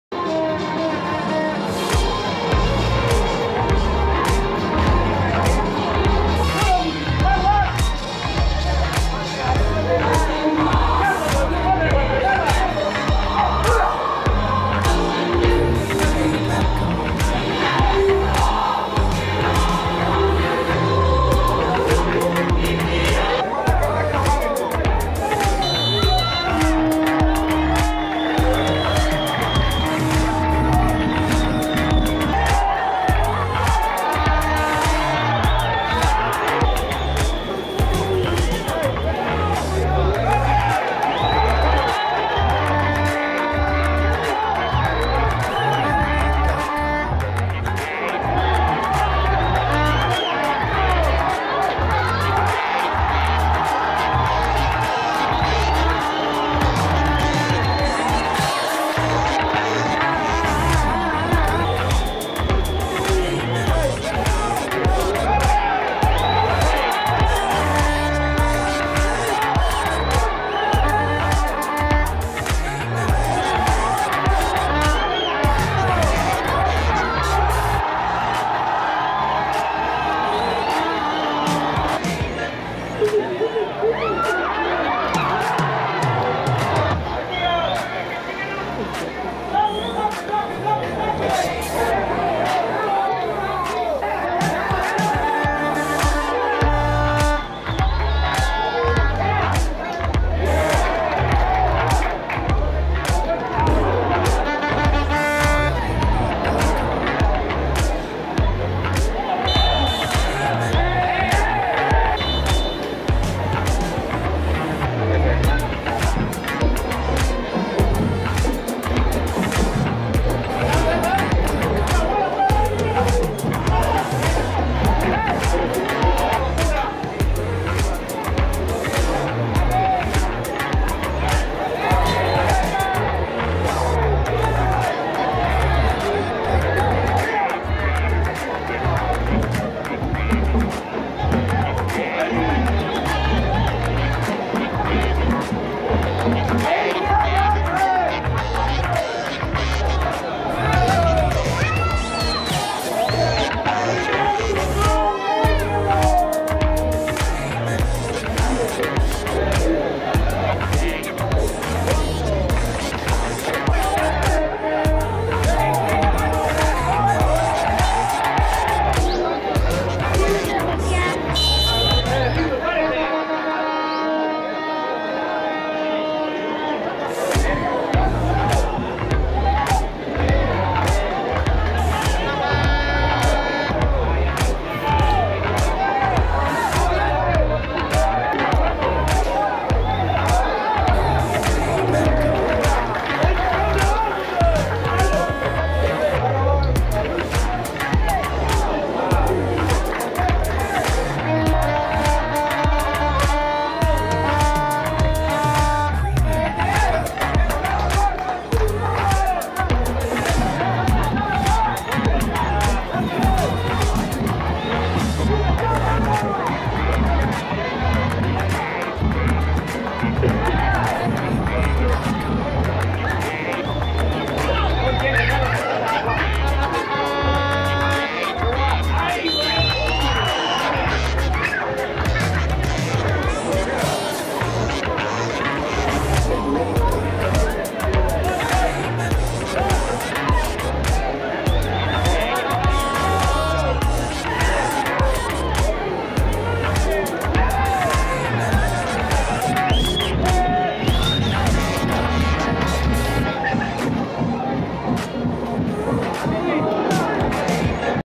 Deportivo Malacateco 1 - Cobán Imperial 0 Jornada 6 Apertura 2022
Deportivo Malacateco 1 - CSD Cobán Imperial 0
Jornada 6     Apertura 2022
Estadio Santa Lucia